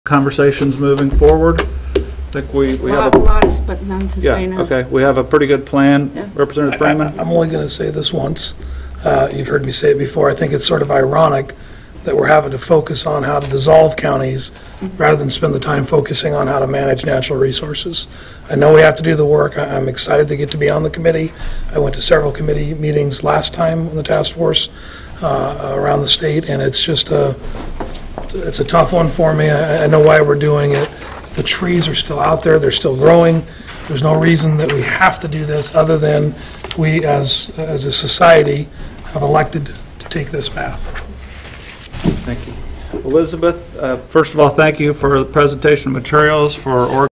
Rep Freeman on Oregon's Rural Counties
Rep. Freeman (R - Roseburg) on timber harvest and the financial challenge facing rural counties in Oregon.